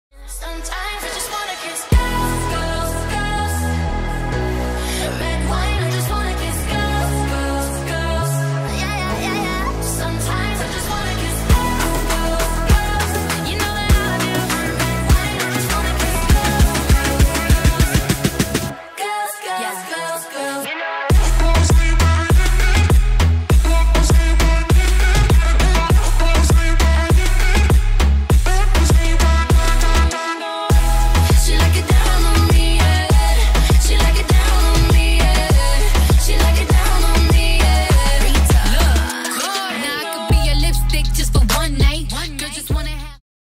MOTHER // ADELAIDE - Launching Saturday 3 November 2K18
MOTHER - Queer Girls Party is Australia's biggest weekly night for queer babes who like to kiss other queer babes.

MOTHER is coming to you ADELAIDE! And we are bringing with us our favourite prisoner from the ever-popular Wentworth television show - Kate Jenkinson aka Allie Novak.